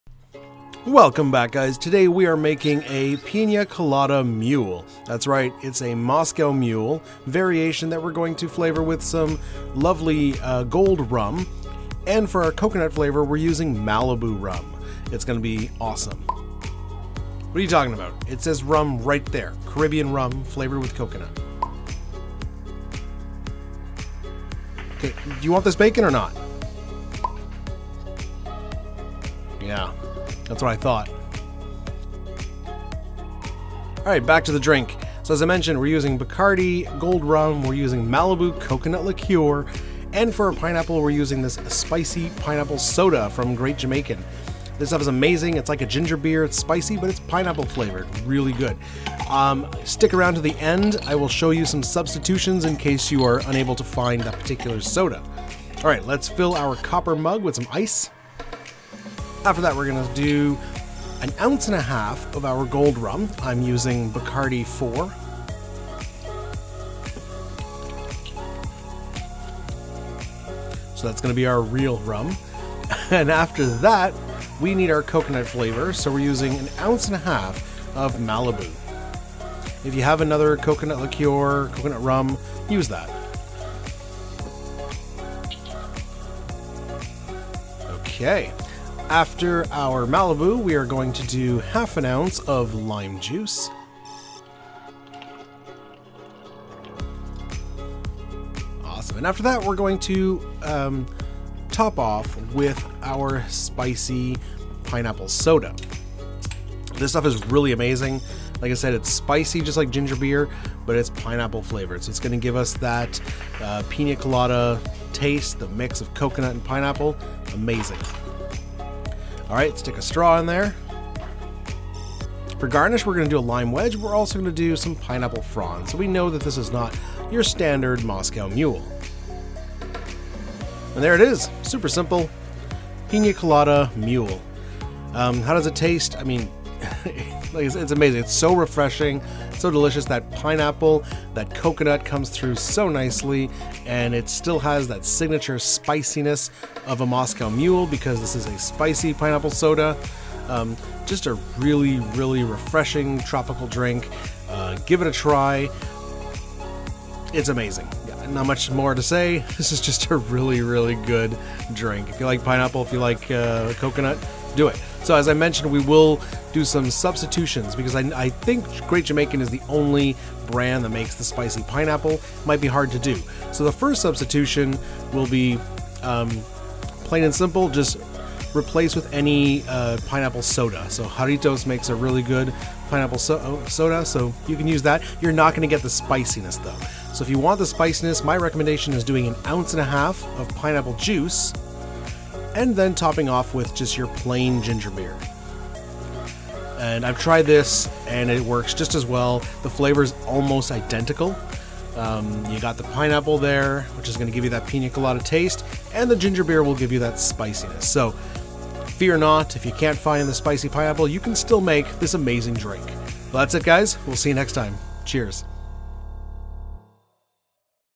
PIÑA COLADA MULE | Moscow Mule Variations | Rob's Home Bar
Piña Colada Mule is a moscow mule variations with Malibu coconut Rum. We're using flavors found a classci Pina Colada to make this ginger beer cocktail, this moscow mule recipe with Pineapple and Coconut.

This is a fantastic Pina Colada Recipe variation. It's a moscow mule with Malibu rum and Spicy Pineapple soda

Pina Colada Mule Recipe:
1.5 oz Gold Rum
1.5 oz Malibu Coconut Rum
1/2 oz Lime Juice
Top with Spicy Pineapple Soda

✅ Let’s Connect!